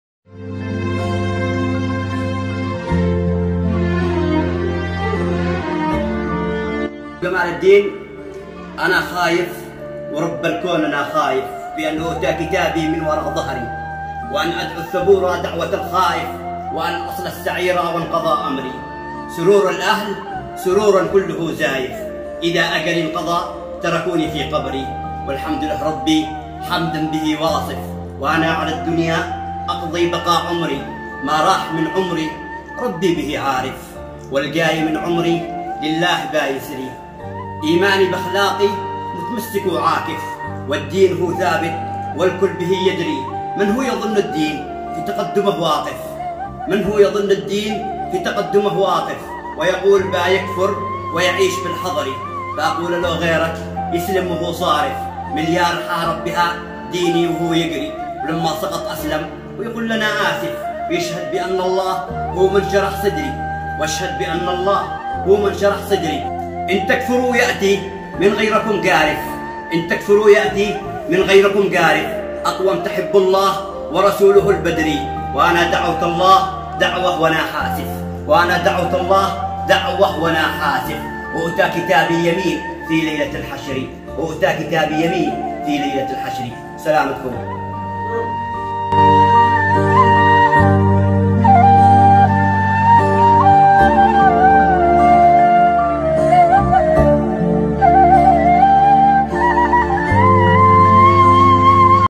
قصيدة انا خايف جميلة جداً وموثرة الشاعر جمال الدين اليماني.شاهد قصيدة قرانيه
‏فيديو من وائل ابو راس